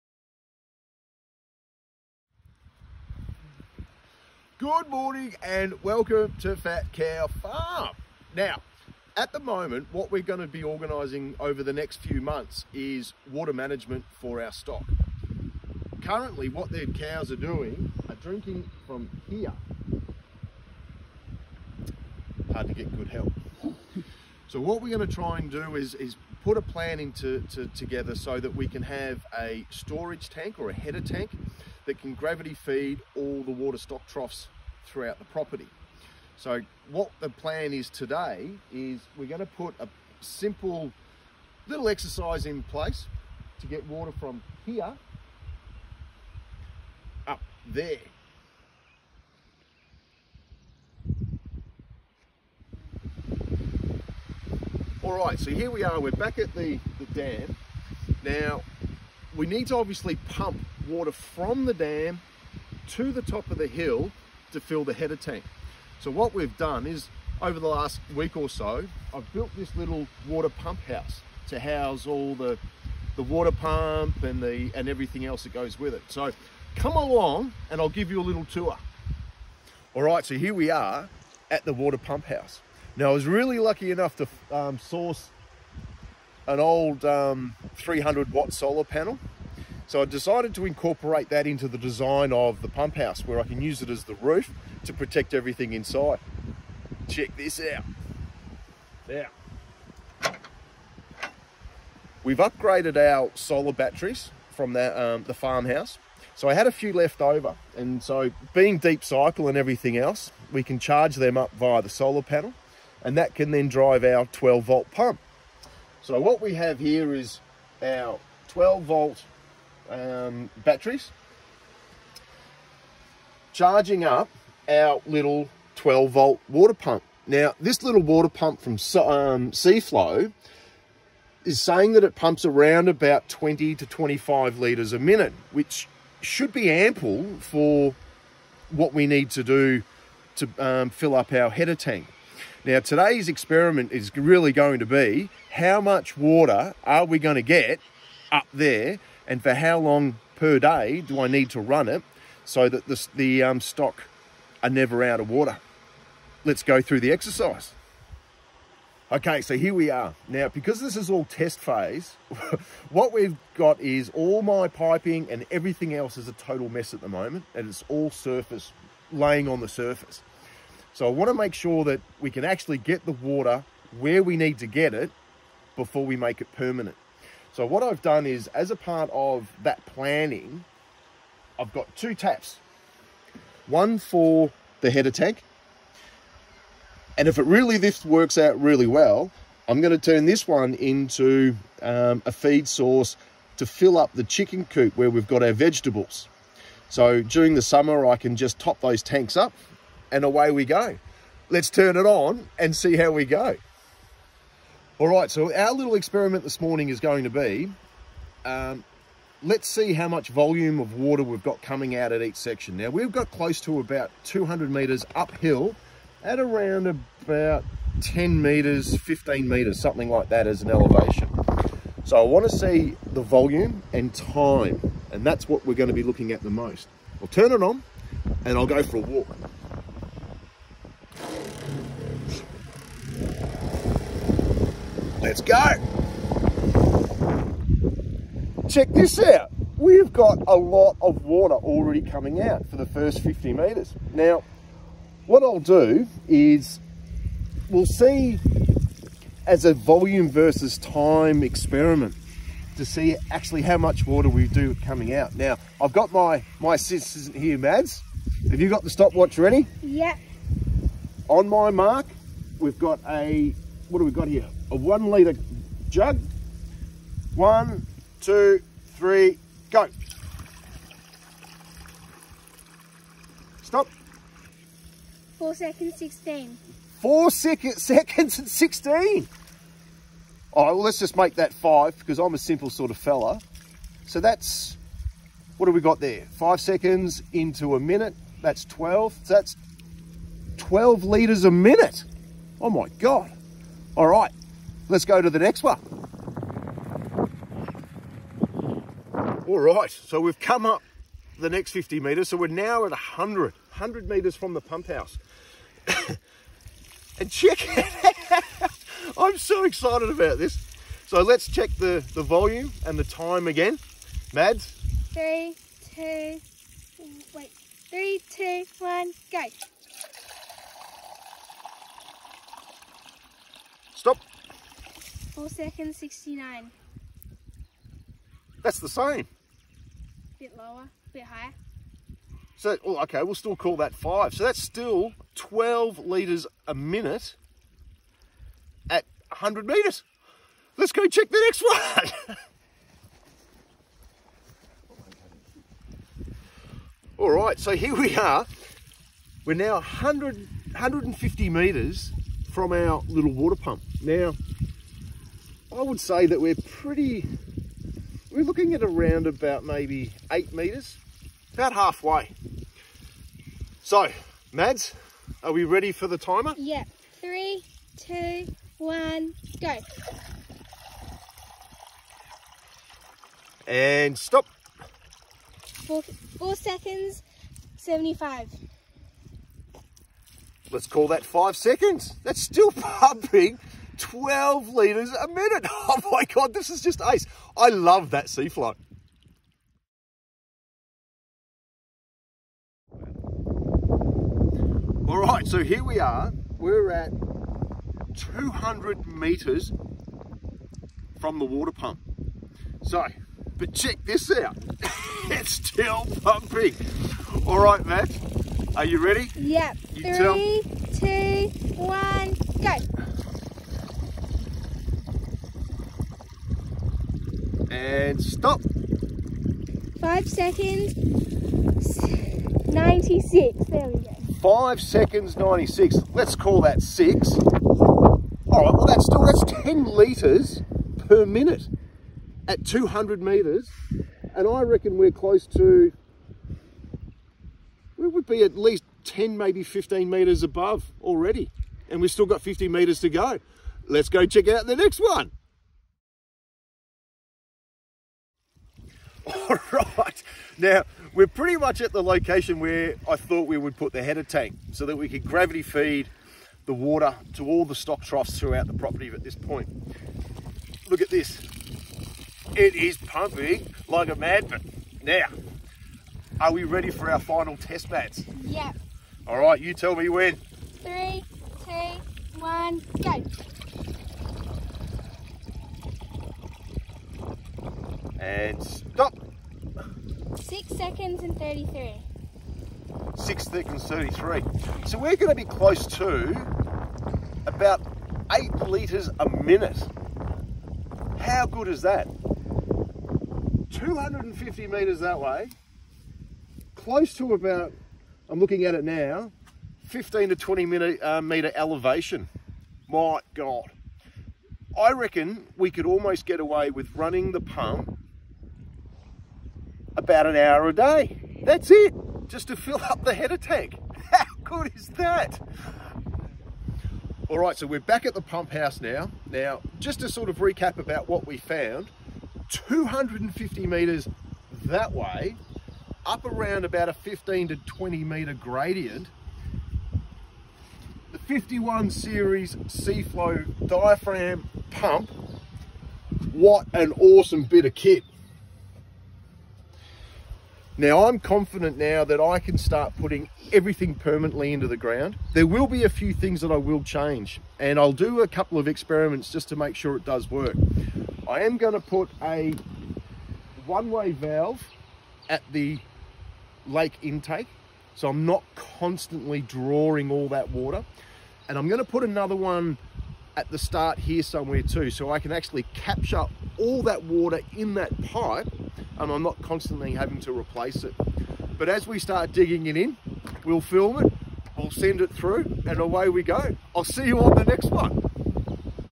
Seaflo Solar 12volt Water Pump
Testing the Seaflo 12volt water pump for farm irrigation.
Water management for the dry summer in Tatong